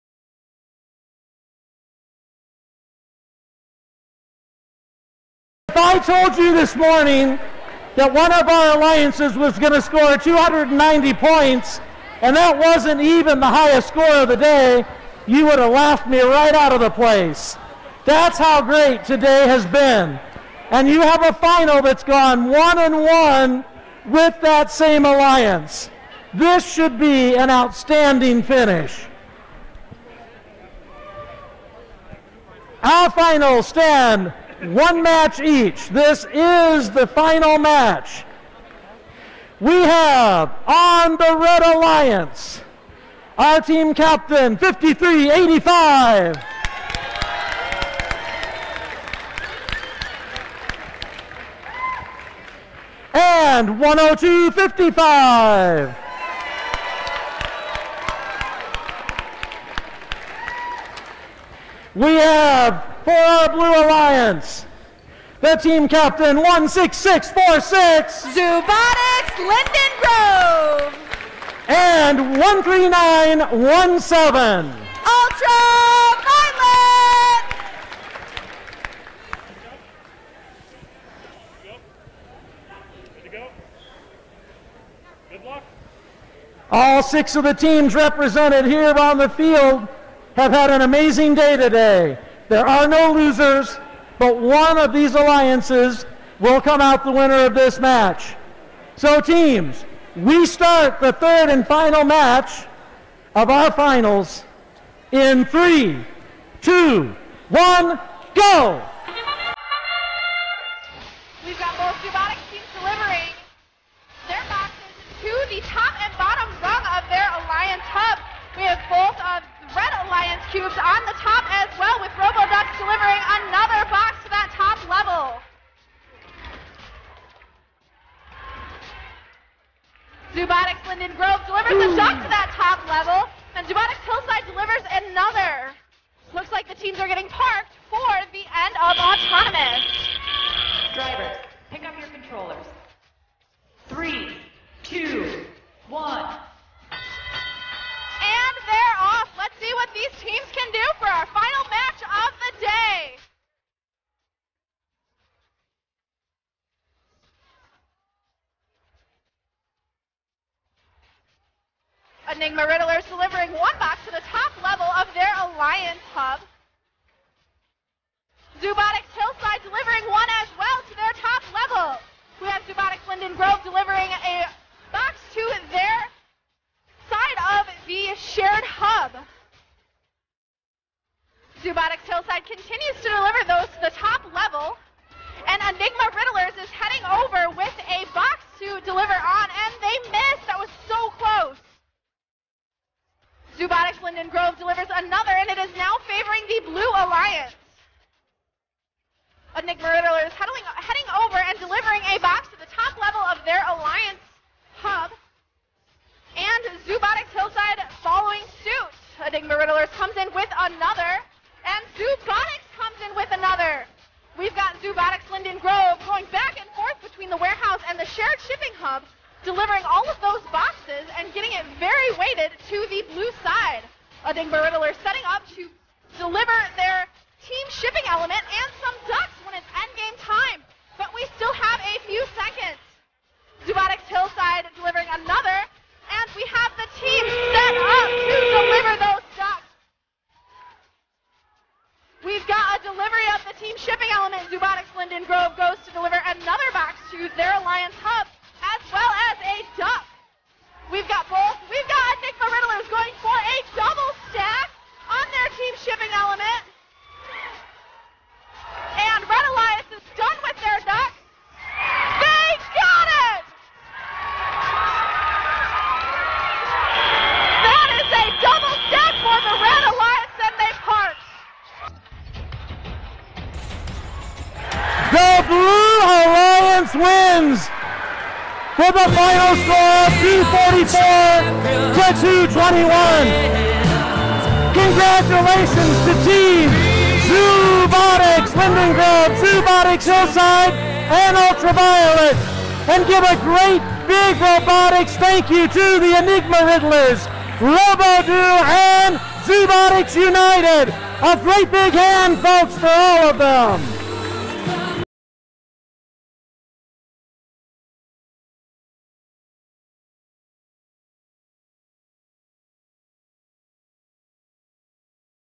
2021-2022 Freight Frenzy FTC 13917 16646 19693 Coloma Final 3 ( 244 vs 221 points) Dec 03-04
FTC# 13917 top left, FTC# 16646 bot left
FTC# 10255 top right, FTC# 5385 bot right

The ZooBotix robots are up against two power house robots who just set a new world record.

ZooBotix 16646 camera in auto, is trained to recognized a green team element. There is a huge green wall across the field which cause the robot to always select level 2.  To minimize the effect, the robot was assign to do the lower points auto and it missed the bonus. It also cause problems in Teleop because the two robot's starting position is now reversed. We should have called a timeout and re-calibrate the camera to use our backup blue colored team element .

We gave everything we had for this win.

Our duck lady's prayer, on bended knees and fingers crossed looking toward the sky, to the FTC gods was answered!